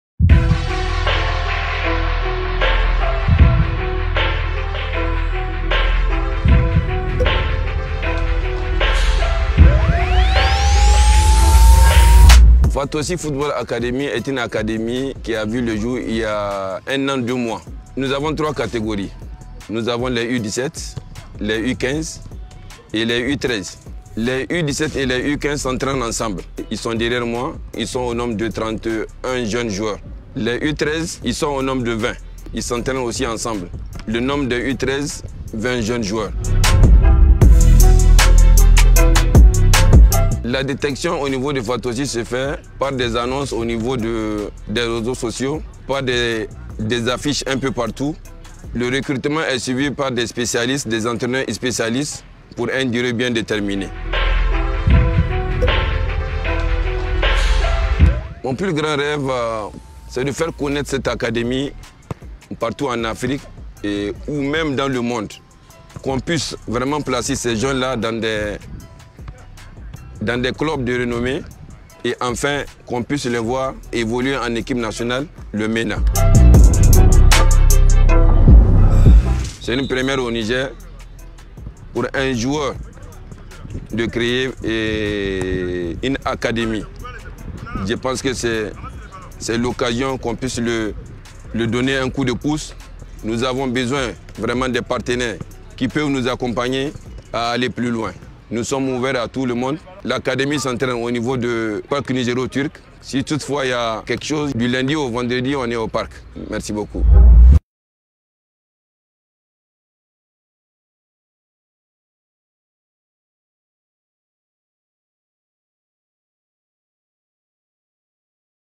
Film documentaire réalisé par @sos-shootingofficiel2108 et
Fatossi Football Academy est L’une des plus grandes Académies sportif professionnelles du Niger 🇳🇪 ,que nous accompagnons il y’a presque 1an.
    ➡️Nous n’avons pas hésité a accompagner ce projet, fondé par un joueur Nigérien du Nom de Yac Diori qui excelle à l’international et à l’équipe Nationale le Mena catégorie A.
   ➡️Il créent le clubs avec ses frère amis pour permettre à des jeunes Nigériens passionnés du football a exercé leur passion
Nous avons tendus le Micro 🎤 au Directeur Technique 👨🏾‍💼 du Clubs MR Daouda Kassaly ancien gardien de but 🥅 de l’équipe nationale Mena,qui nous parle de l’évolution du Clubs